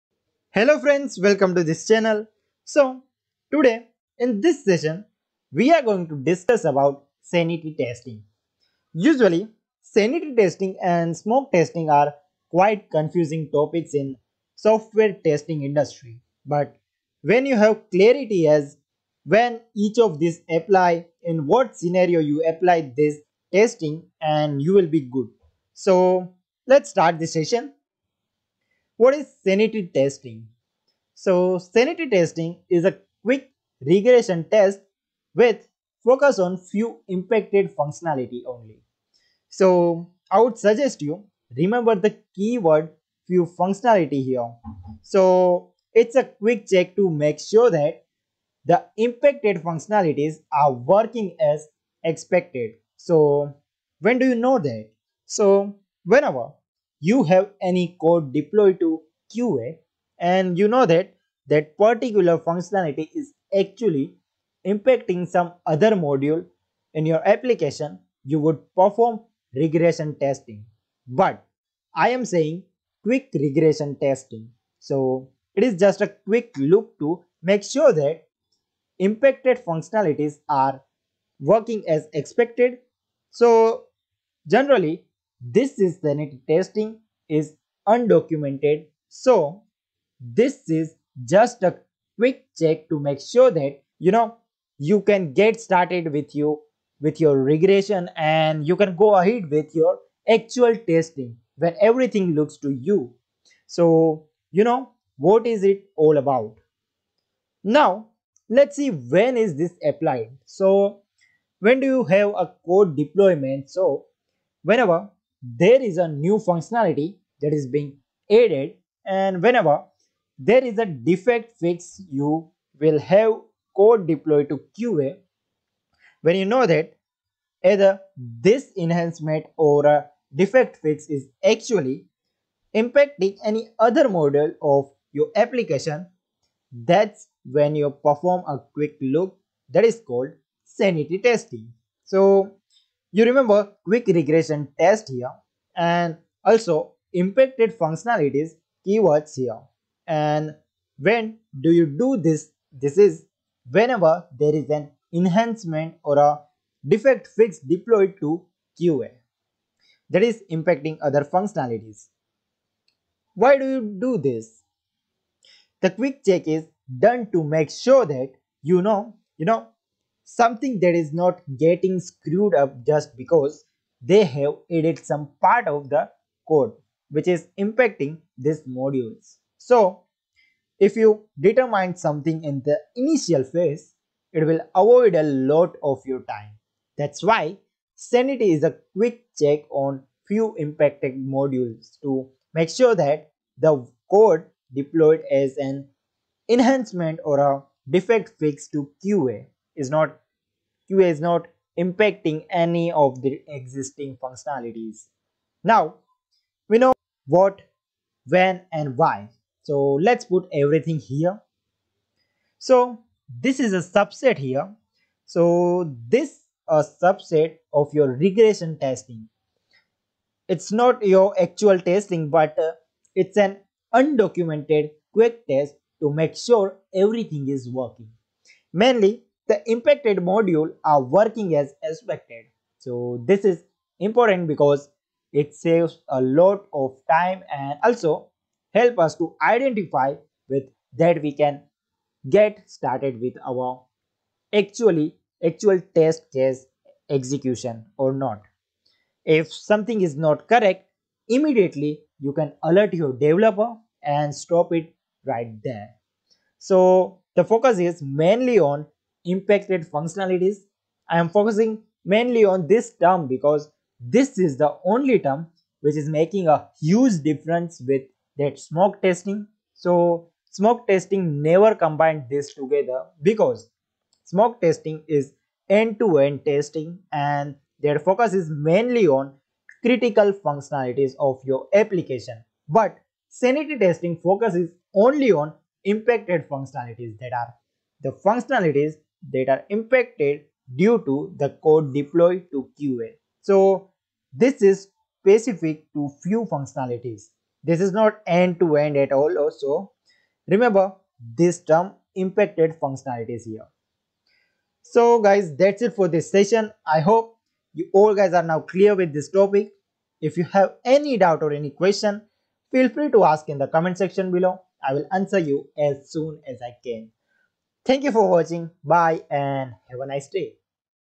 What is Sanity Testing | Quick Overview | Software Testing Tutorial | Easy Explanation
This video will help you get in-depth knowledge of sanity testing and how sanity testing helps find bugs in the early stages of testing.